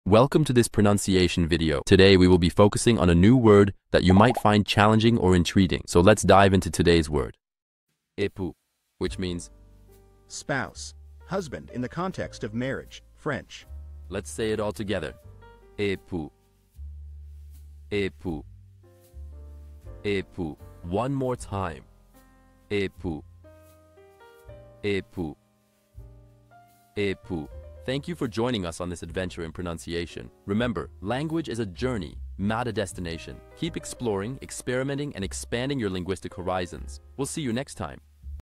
How to pronounce Époux
Master the Pronunciation of 'Époux Which Means Hp' - which means : Spass;Husband in the Context of Marriage (French). 📝 with @BaguettePronunciation 🎓🗣️ - [Your Guide to French]

Learn to pronounce 'Époux' perfectly in French with our easy and engaging tutorial by @BaguettePronunciation! 📖📽️ In this comprehensive video, we provide you with native speaker examples 🗣️👂 and phonetic breakdowns 🎵 to ensure you get the pronunciation just right.

How to say Husband in French